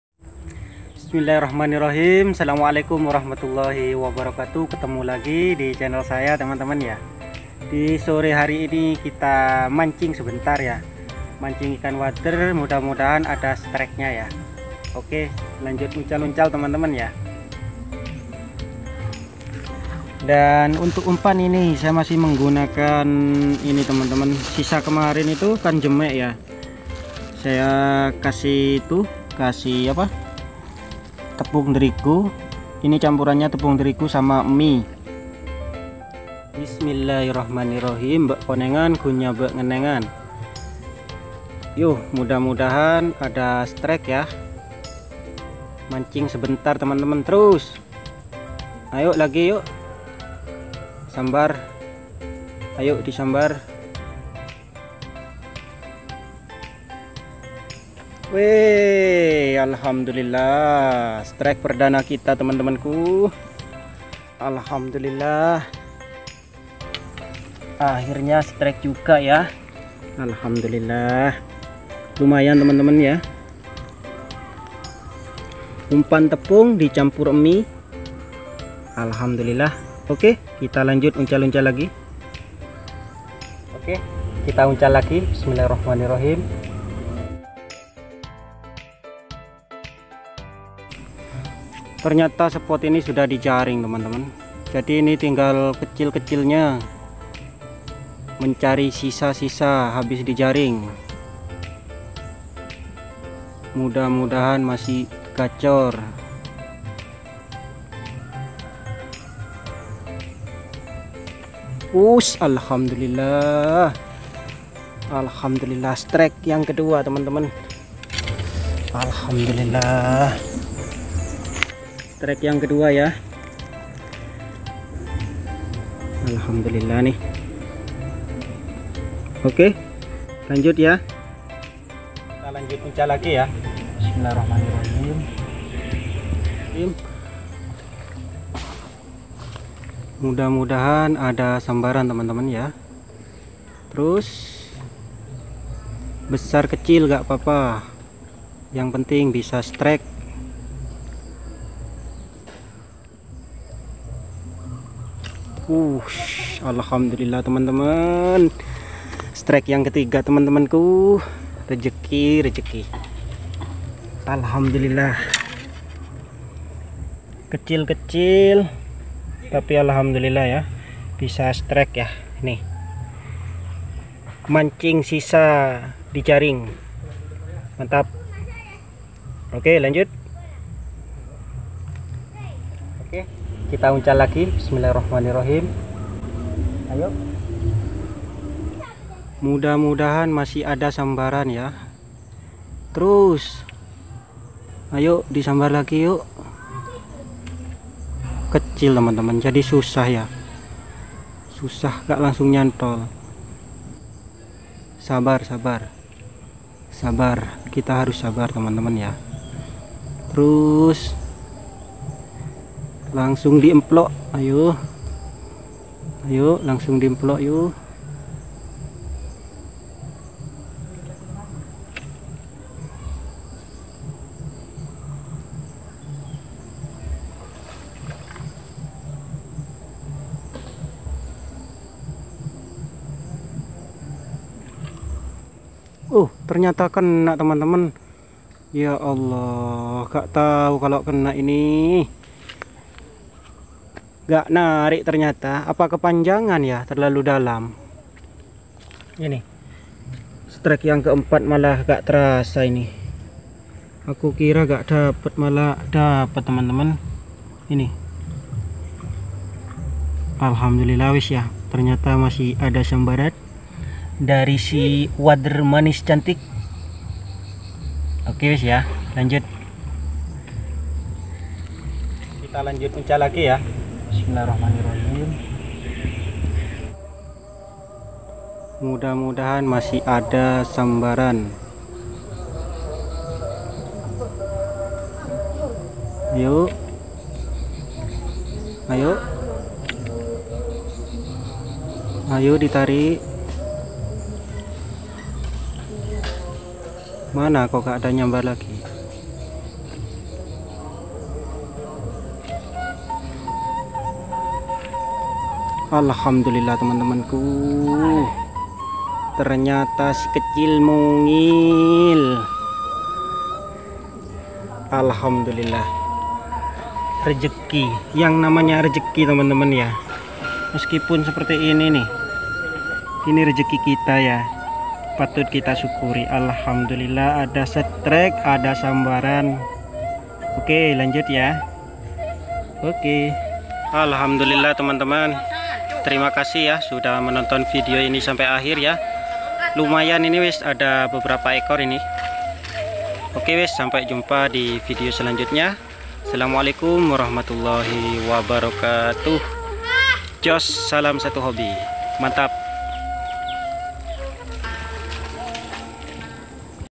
Mancing Liar Ikan Wader Di Kira Sudah Habis Ternyata Masih Banyak
Mancing Liar Ikan Wader Di Kira Sudah Habis Ternyata Masih Banyak Ikannya Sobat,,, karena kemarin ada yang menjala...
Semoga video ini bisa menghibur sobat Semua Amiiin🙏🙏🙏